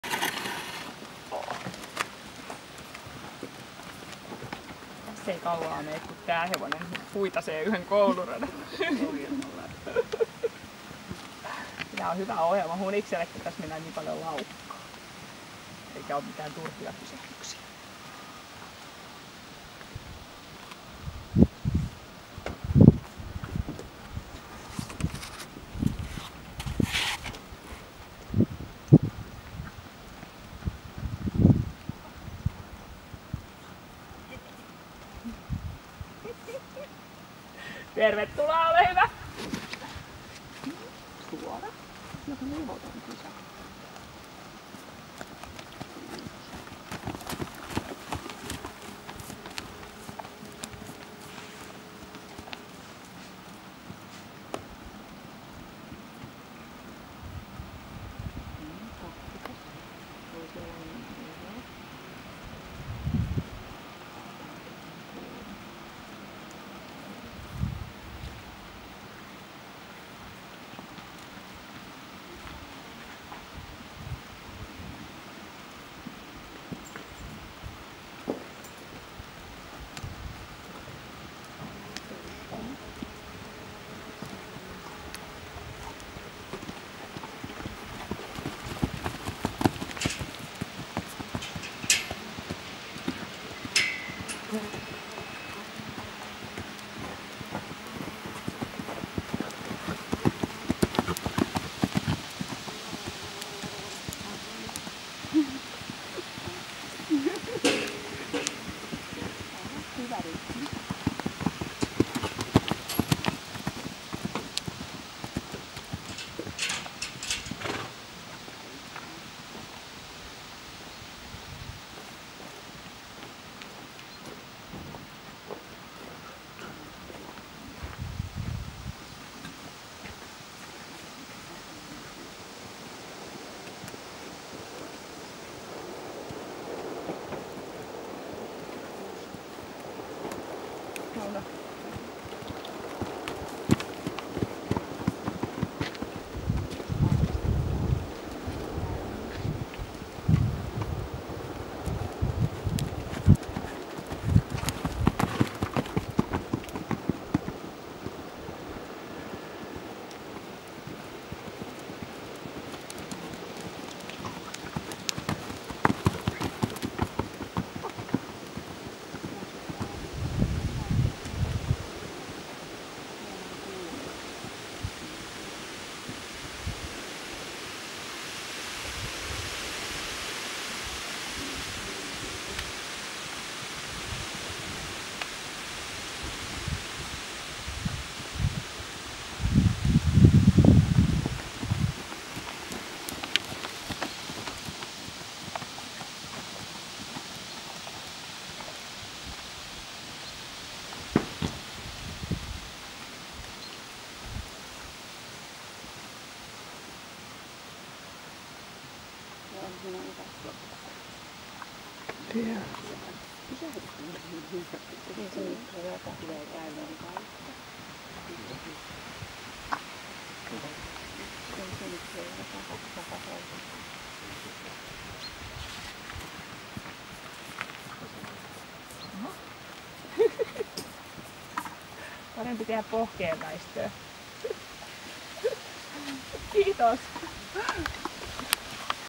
C-merkin kouluohjelma
Elämäni ensimmäinen c-merkin kouluohjelma ratsastettu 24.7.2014 Saajan tallilla, Hämeenlinnassa. Hevosena 5-vuotias Hunajakaneli.